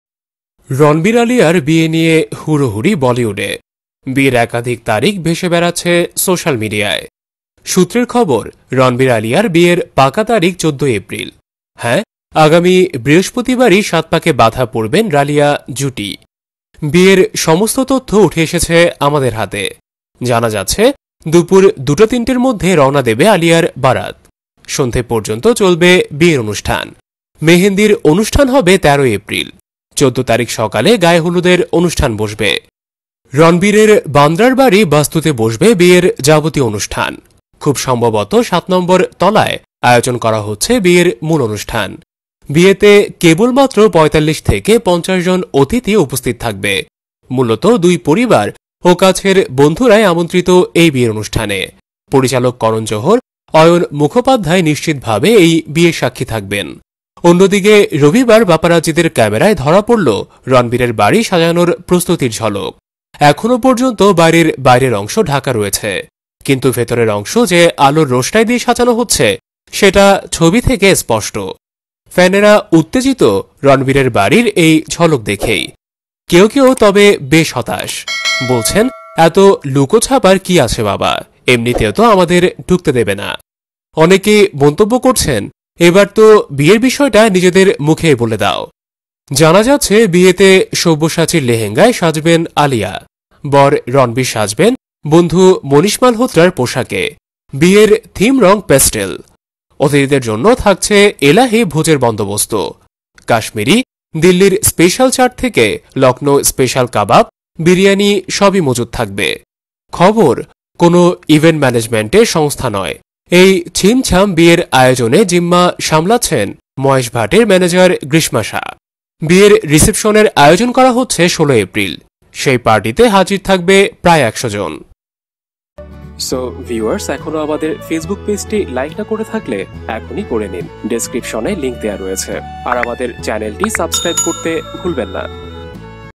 রণবীর-আলিয়ার বিয়ের পোষাক থেকে খাবারের মেনু... সব খুঁটিনাটি দেখে নিন! | Ranbir & Alia Marriage 2022
Video Details:
Ranbir Kapoor-Alia Bhatt Marriage News 2022